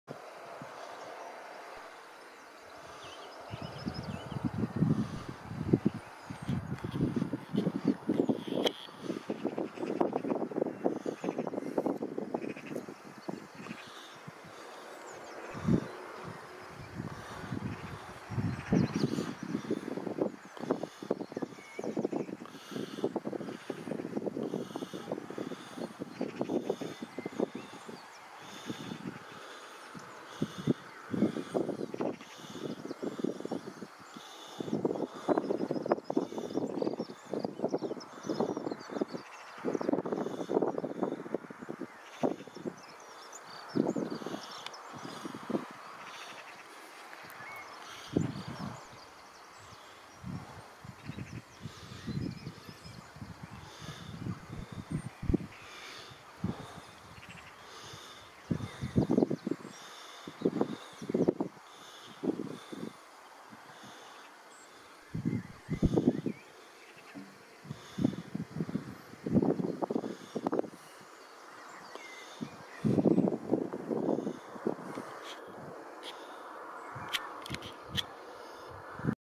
A family of Stoats at Play - Cornwall Part 1
Lucky enough to catch a family of Stoats at play in one of our fields in Wendron, Cornwall on 13th June 2020